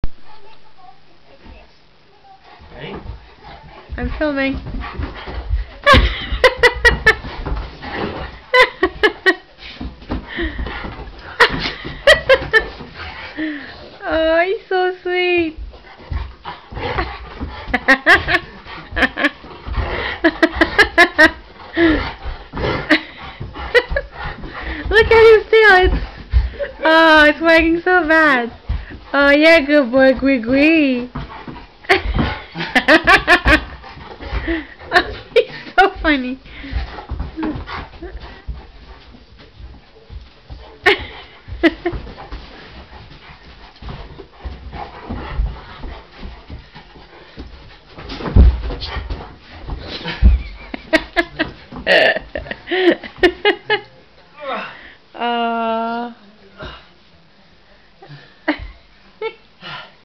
Grizzly time
Darcy and Grizzly having fun Saturday morning...